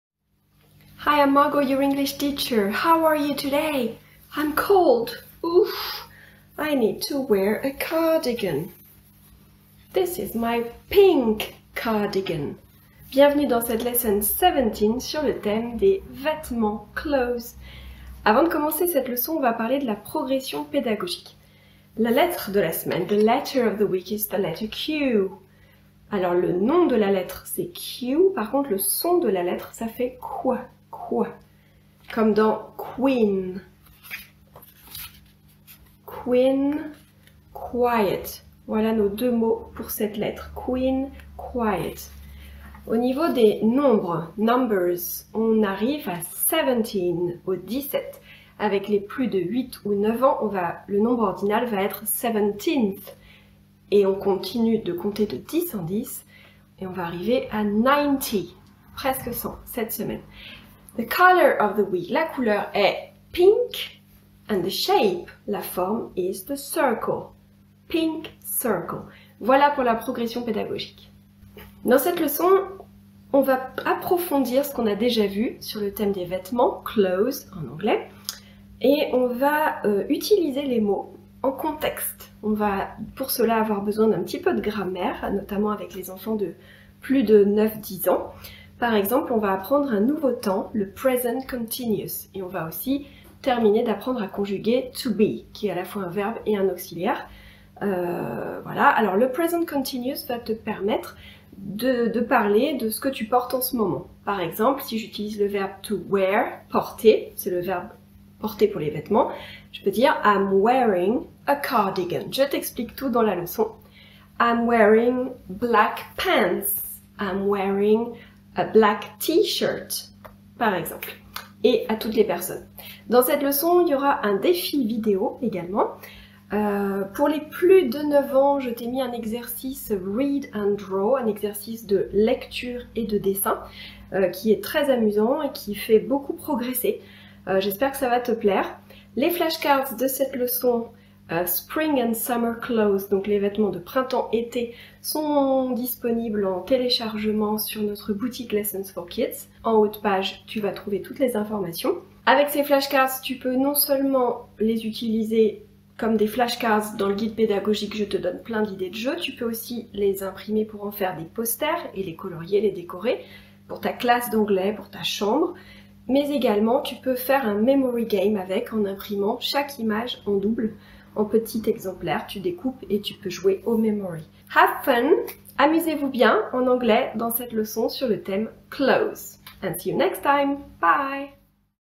Lessons4Kids Lesson 17 Introduction - Clothes (2)
Bienvenue dans la "Lesson 17" de la formule de cours d'anglais en ligne hebdomadaires Lessons4kids !

Ces cours ludiques et créatifs sont conçus par une professeure d'anglais expérimentée, pour un public d'enfants de 3 à 11 ans.

Dans cette dix-septième leçon d'anglais, nous approfondissons la leçon 7 sur le thème des vêtements, selon l'âge des enfants.

Beaucoup d'activités ludiques, de ressources imprimables et un défi vidéo à réaliser pour encore + de fun :)

Suivez-vous sur Instagram @lessons.books4kids